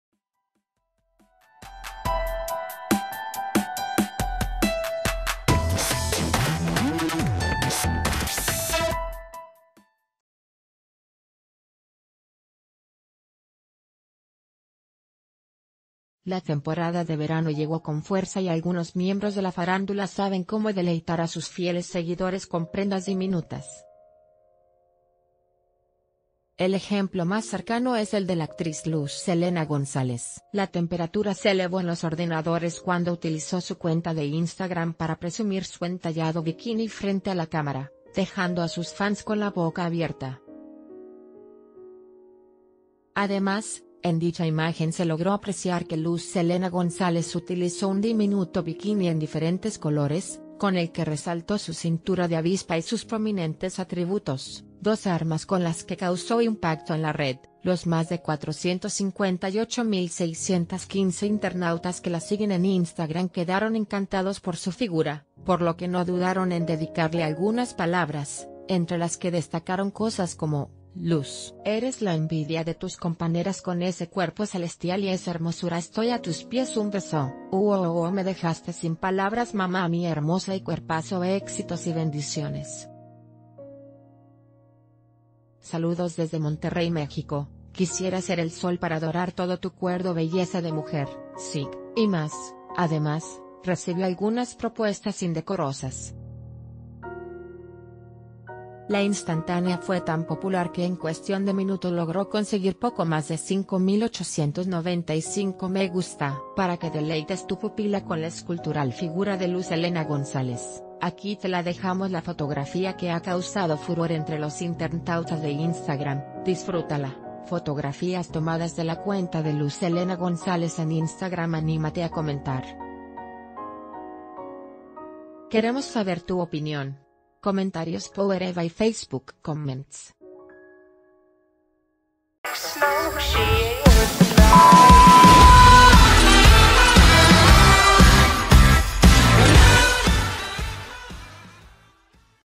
Luz Elena González desparrama sus atributos en diminuto bikini (FOTO)
La temporada de verano llegó con fuerza y algunos miembros de la farándula saben cómo deleitar a sus fieles seguidores con prendas diminutas. El ejemplo más cercano es el de la actriz Luz Elena González. La temperatura se elevó en los ordenadores cuando utilizó su cuenta de Instagram para presumir s...